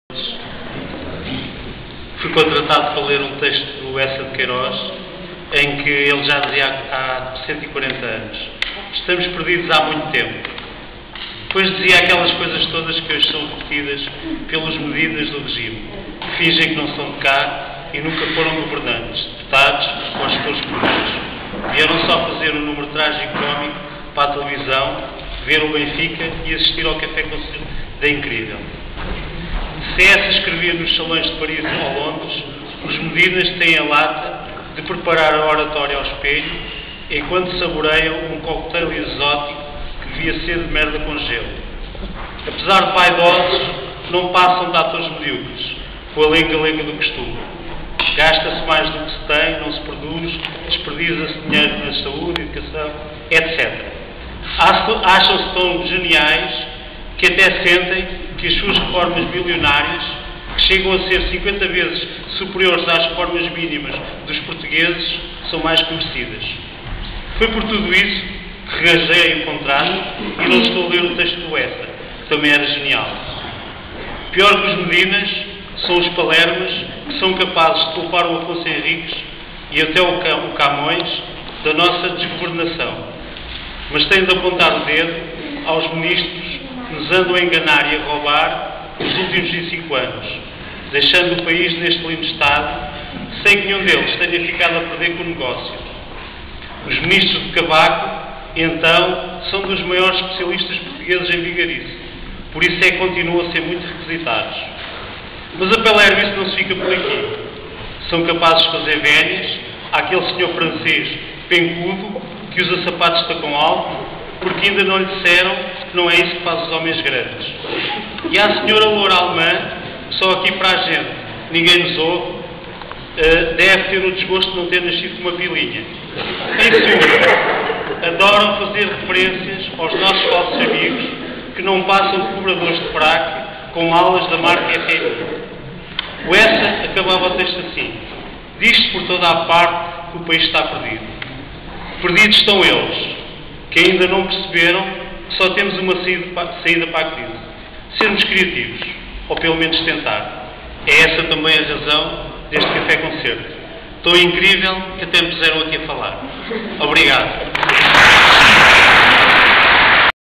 Luis Milheiro
Participação de Luis Milheiro, Vice-Presidente cultural, Recreativo e Desportivo da SFIA, com um texto de sua autoria, no café-concerto de 3 de Dezembro de 2011, no salão de festas.